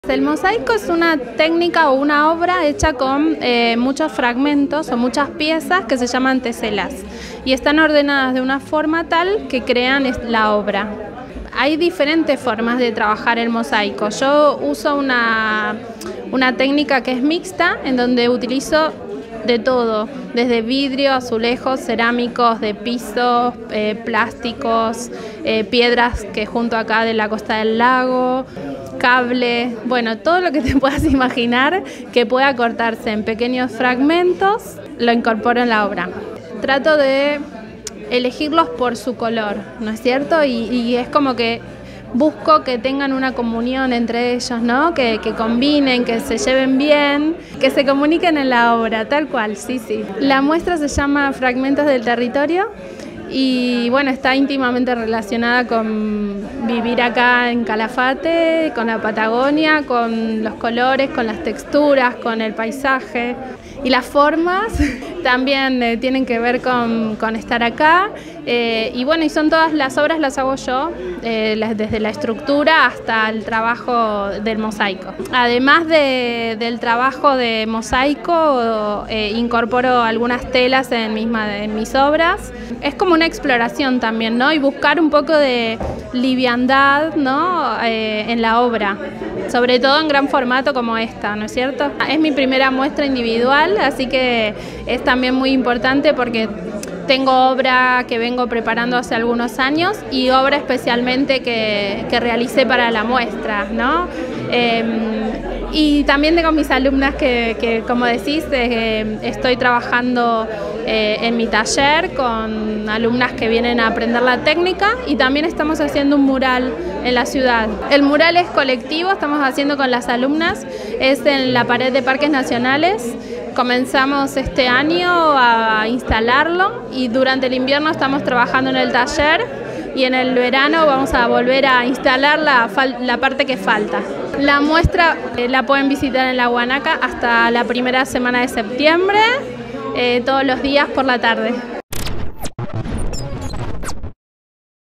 La Artista Plástica Alejandra Martín presentó su muestra de Mosaico
Este sábado por la tarde se inauguró la muestra "Fragmentos del territorio" con la técnica mosaico, de la Artista Alejandra Martin de ElCalafate.

La misma permanecerá vigente hasta la primera semana de septiembre en la Galería de Arte local La Guanaca Azul.